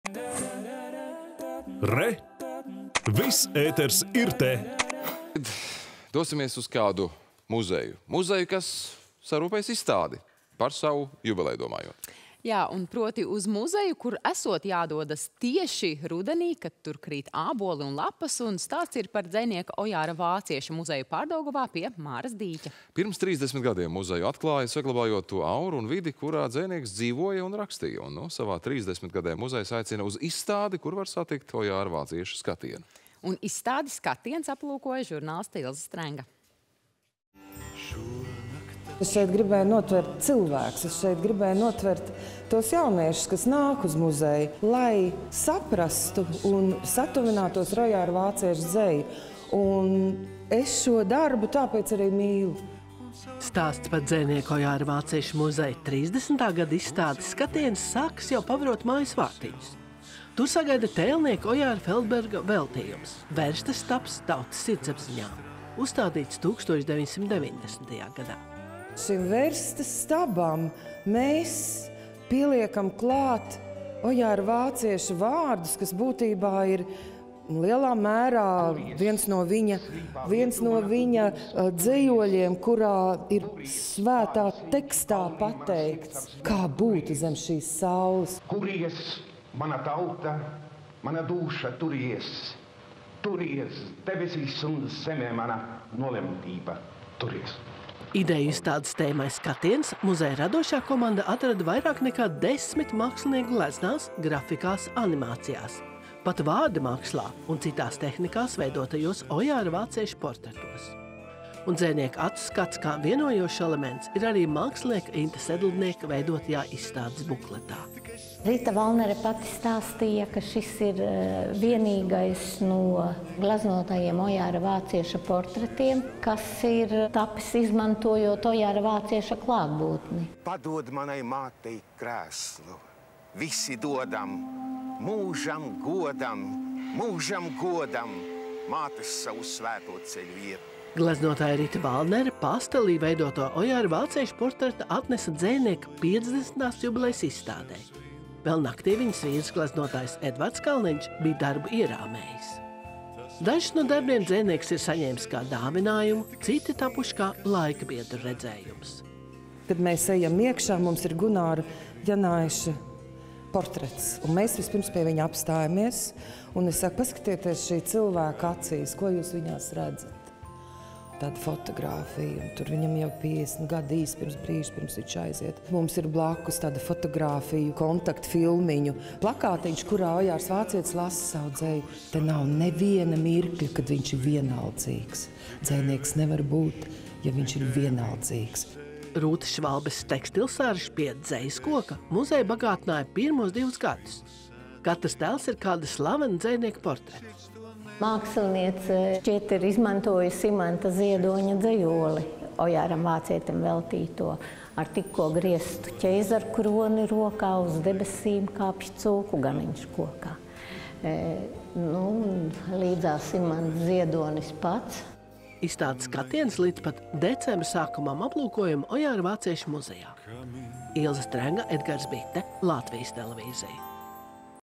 Ojāra Vācieša “Skatiens” - muzeja trīsdesmitgadē
Uz dzejnieka Ojāra Vācieša muzeju  Pārdaugavā pie Māras dīķa jādodas rudenī – kad viņa dārzā  krīt āboli un rudens lapas.
Pirms trīsdessmit gadiem muzeju atklāja, saglabājot to auru un vidi, kurā dzejnieks dzīvoja un rakstīja. Trīsdesmitgadē muzejs aicina uz izstādi, kur var satikt Ojāra Vācieša skatienu.
    Izstādi “Skatiens” aplūkoja  žurnāliste Ilze Strenga.